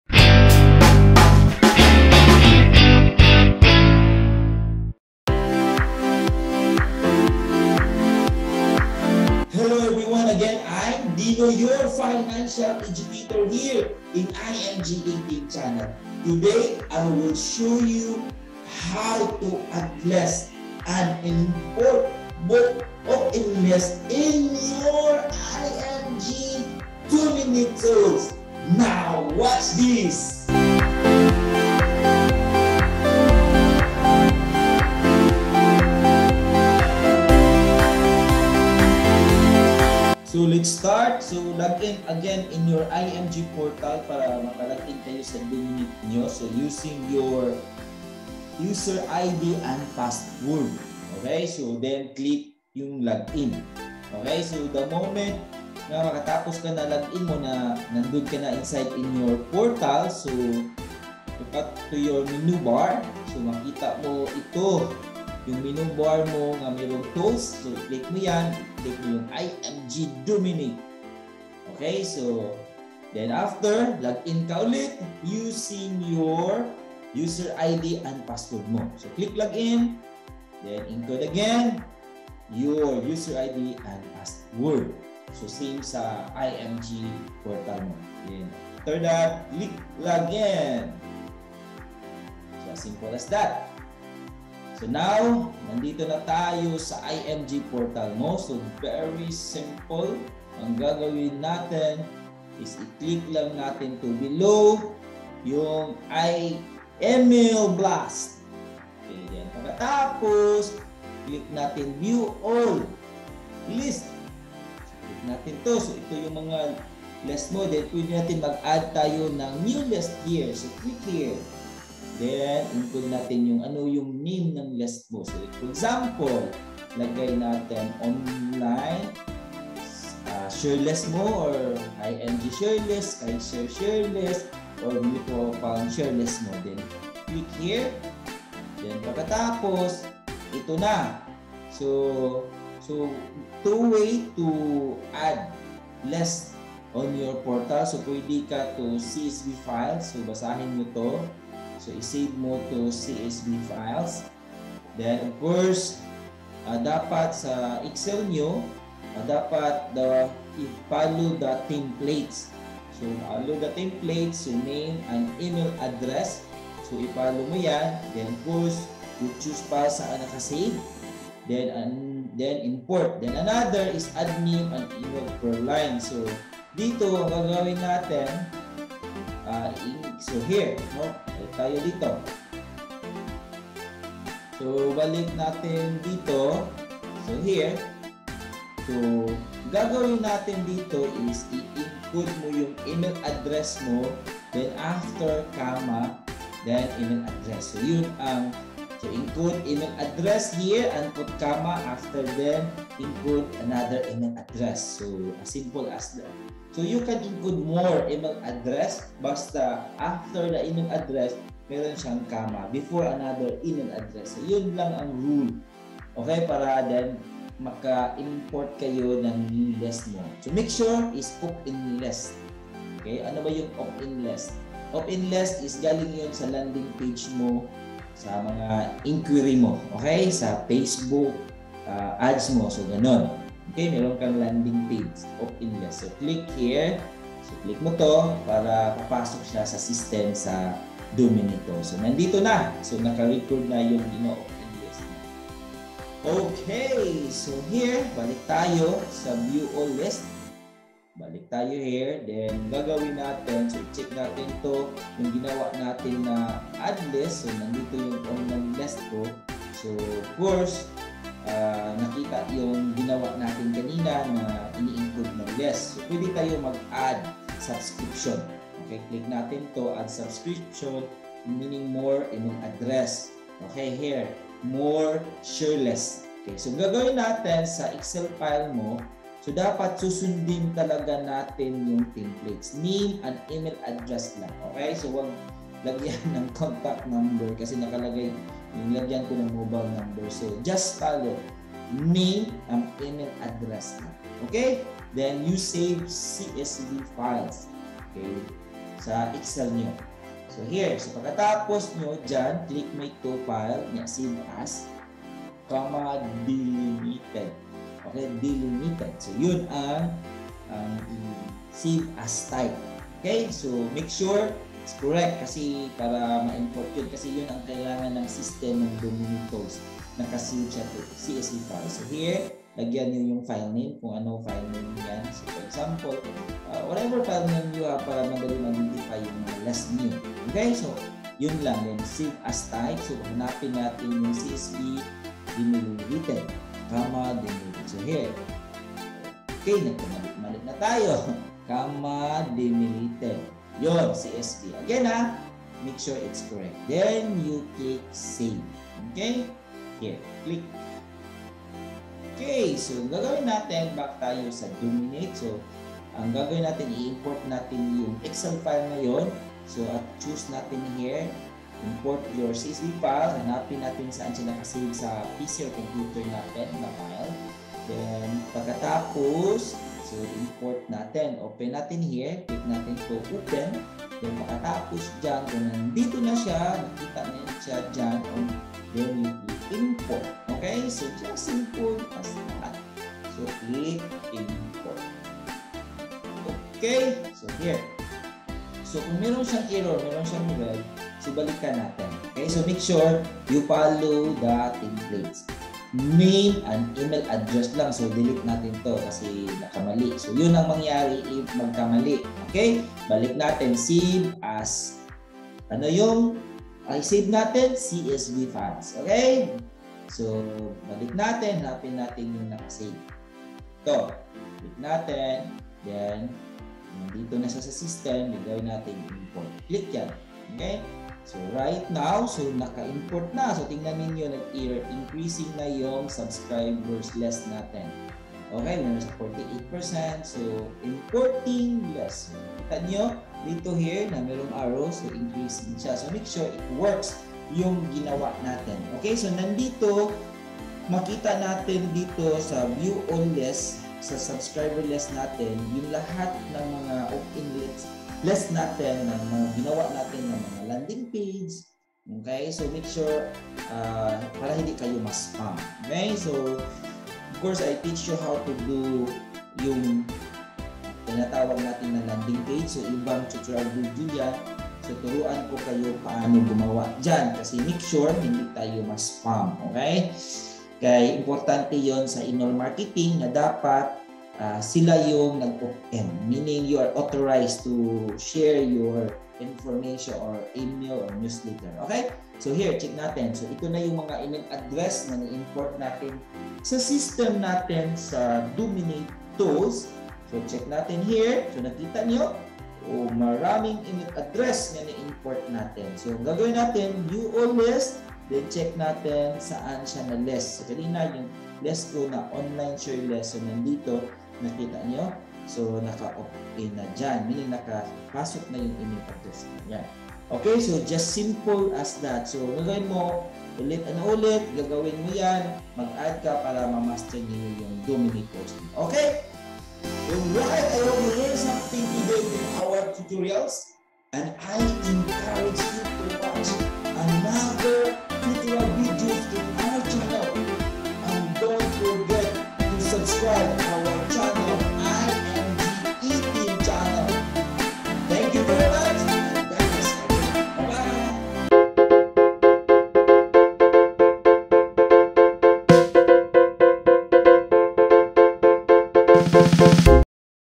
IMG Dominate Tools Class 2 - How To Add List and Import Bulk Opt-in List in your Dominate Tools
This Video will guide a new IMG Members in How To Add List and Import Bulk Opt-in List in your IMG Dominate Tools.